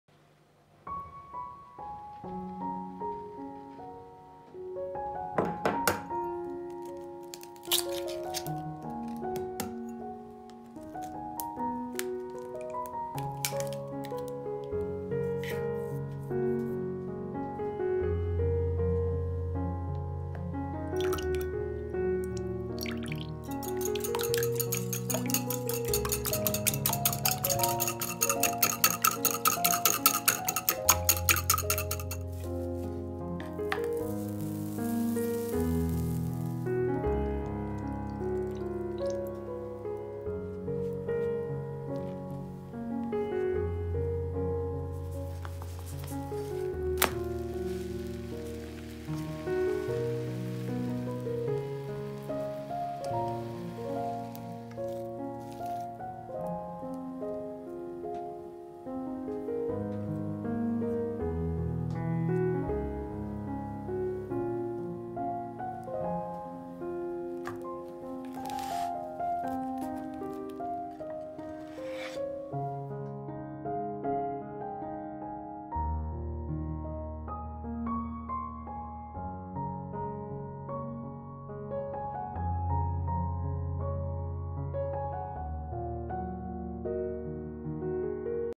Double Cheese Toast
• 2 eggs
• toasted bread 2 pcs
• toasted cheese 2 pcs
• smoked chicken fillet 2 pc
Method of cooking:
Beat the egg with milk, add salt and mix well. Pour the oil and egg mixture into the heated pan. Fry on low heat for 2-3 minutes under the lid. Put two pieces of bread on the egg and fry on both sides. Spread toasted cheese, salami and wrap with the second half of the toast.